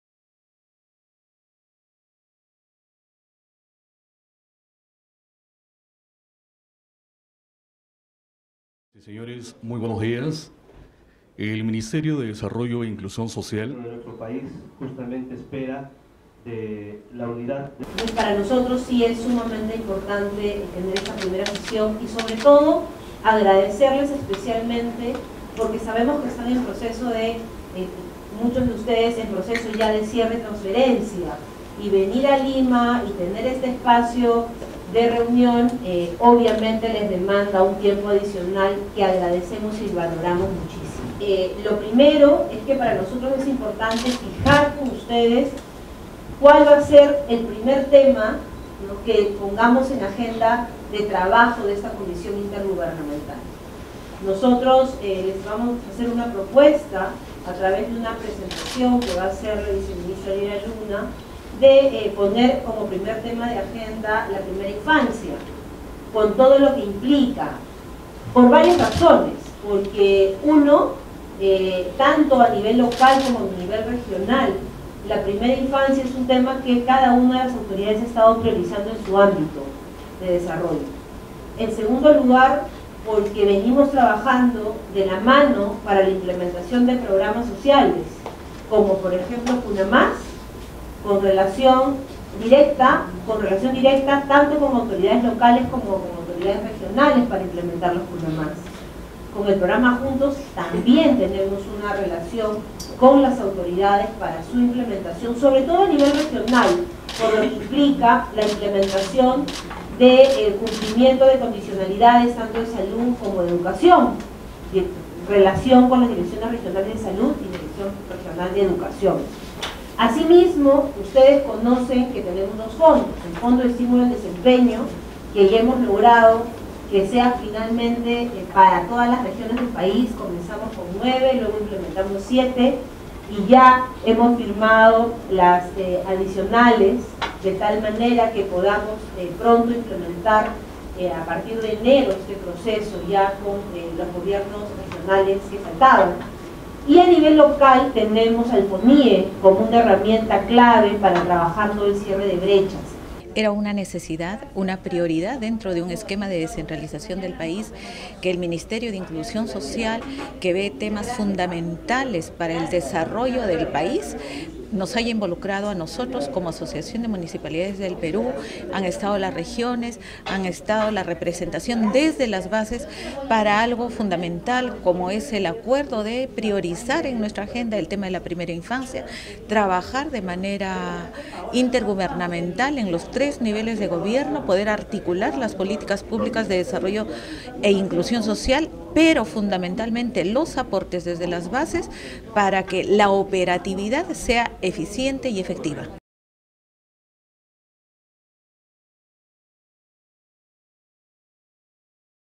Primera infancia será prioridad en comisión de desarrollo e inclusión social
El desarrollo infantil temprano será tema prioritario para el Gobierno Nacional, los gobiernos regionales y los gobiernos locales, cuyas autoridades participaron este jueves de la instalación de la Comisión Intergubernamental de Desarrollo e Inclusión Social.

La ministra de Desarrollo e Inclusión Social (MIDIS), Paola Bustamante, preside dicho grupo de trabajo en donde los diferentes niveles de gobierno implementarán políticas públicas y definirán compromisos concretos, entre ellos la reducción de la desnutrición crónica infantil.